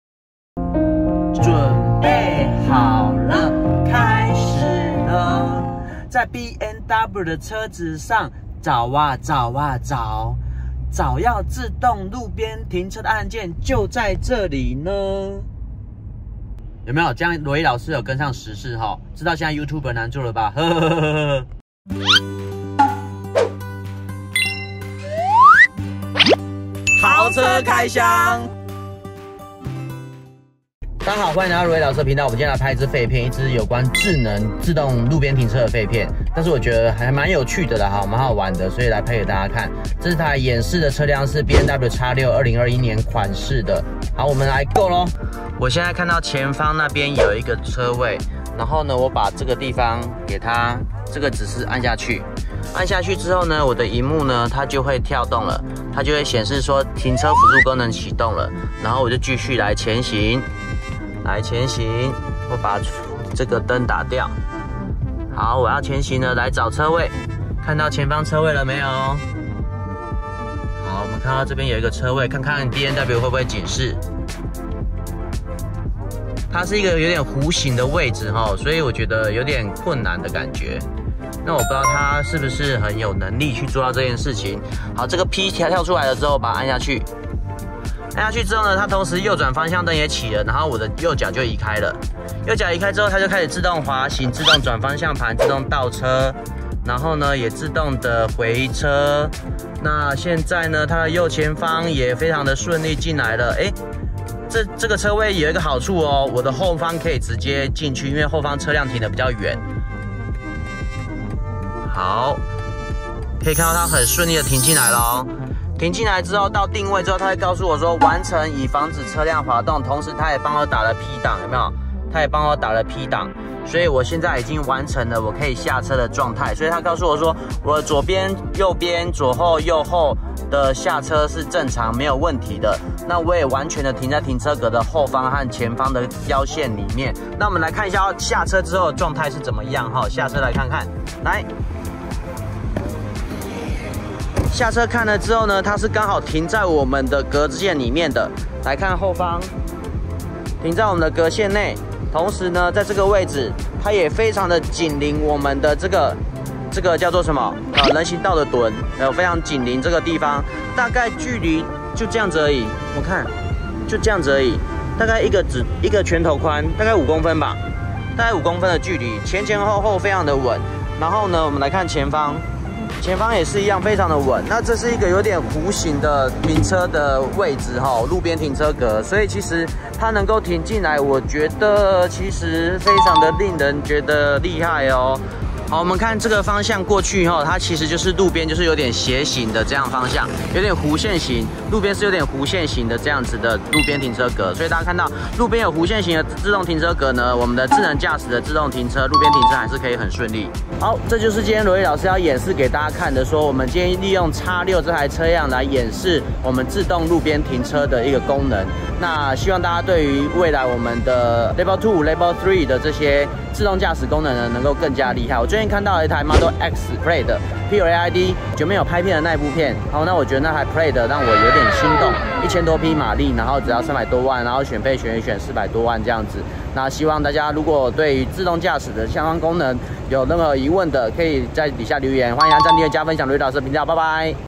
【LEVEL 2道路實測】BMW X6 21年式5AU系統，你是不是也很害怕路邊停車，肥6自動路邊停車實測，明明很靠近路邊，還是壓線了。弧線路邊停車照樣難不倒電腦判斷。 FT. 羅伊老師 EP.21
你還在對於LEVEL 2 有疑慮嗎
我想個廠牌多少會有差別
但是BMW 的 5AU系統
非常值得信賴的路邊停車好幫手
車子再寬再胖
照樣停得進去

拍攝器材：NIKON Z7、I-PHONE14 PRO
收音器材：Rode Pro Plus
剪接軟體：Adobe Premiere

版面LOGO露出募集中